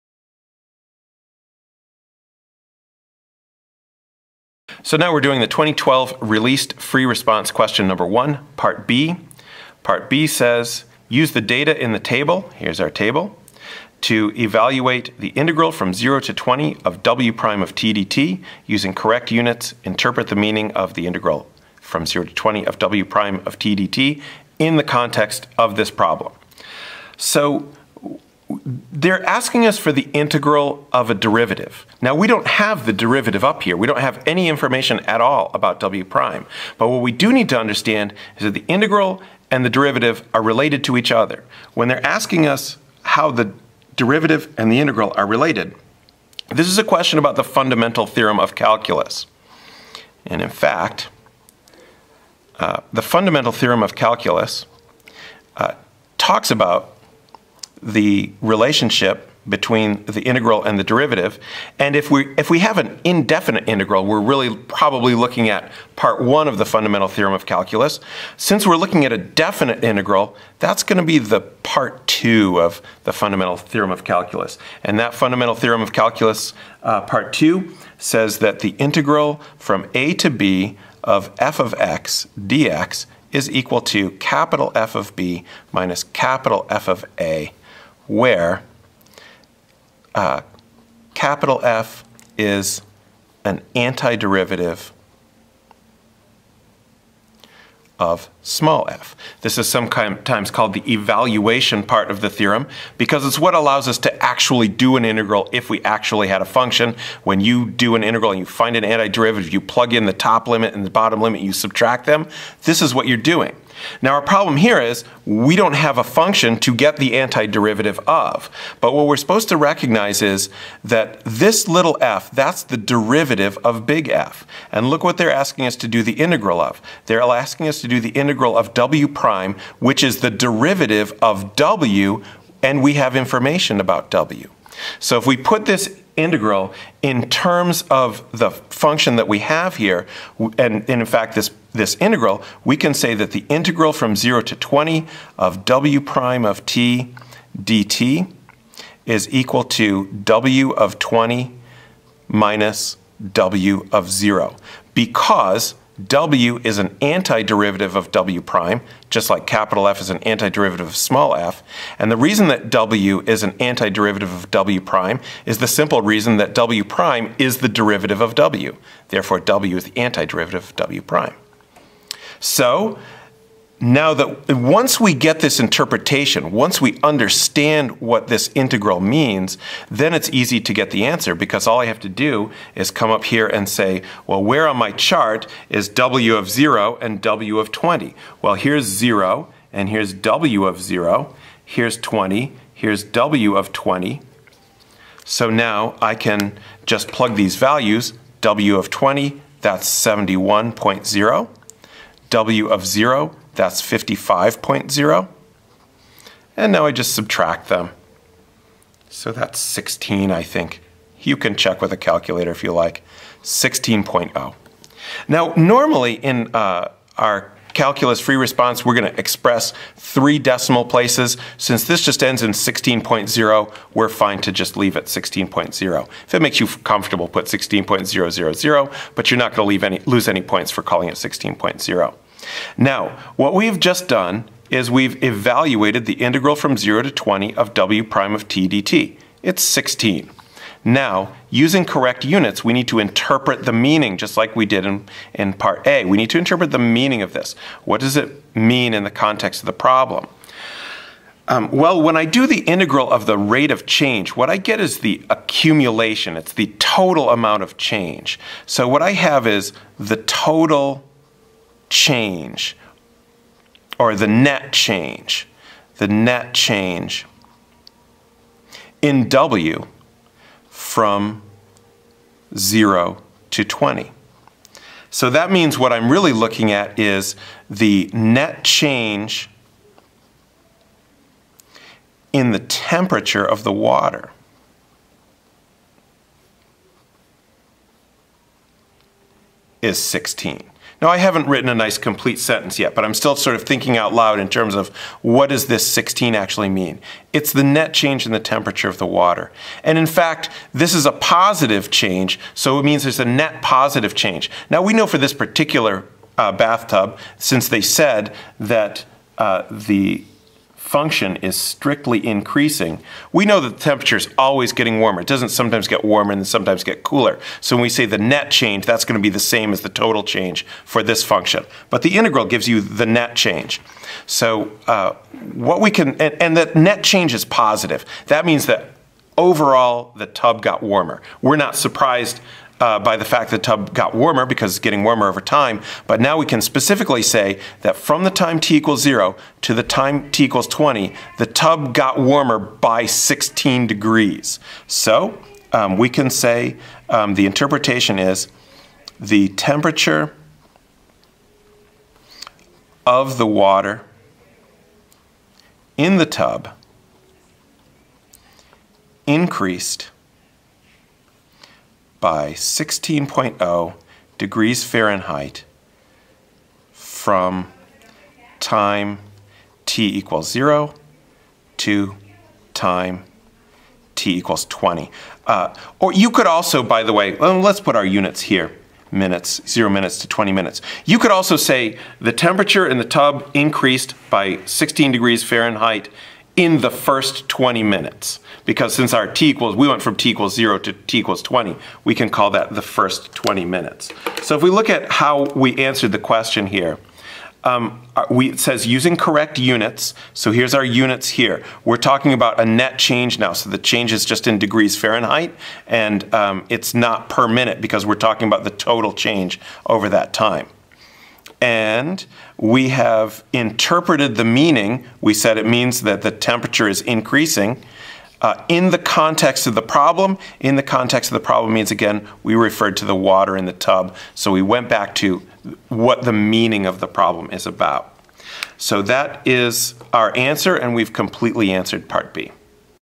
2012 AP Calculus AB Free Response
An explanation of how to complete the 2012 AP Calculus AB Free Response Section 2 Part A Question 1b.